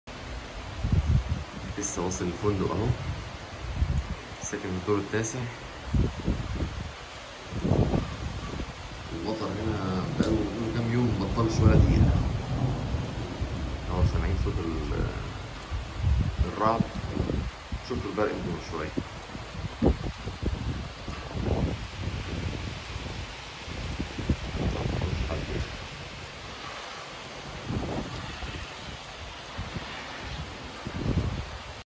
أمطار ⛈ أنطاليا 🇹🇷 من فندق SU
إنذار ٢٦ نوفمبر ٢٠١٨ استمرت ٣ أيام متواصلة ومن بعدها استمرت طوال أسبوع متقطعة ولكن بغزارة